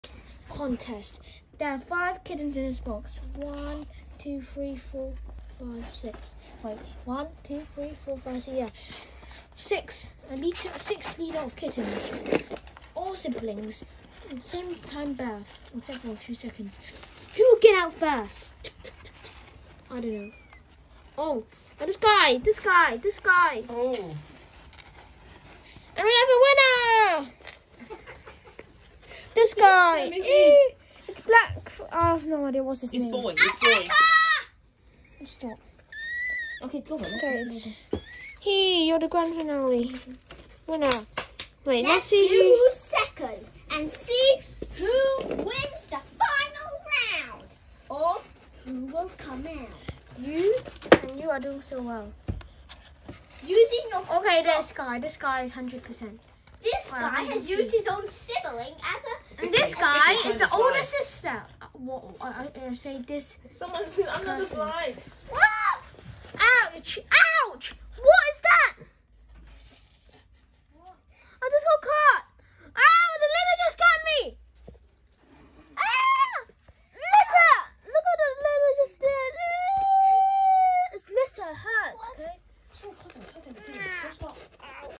Kitten contest
Watch these 6 kittens escape a mini littering box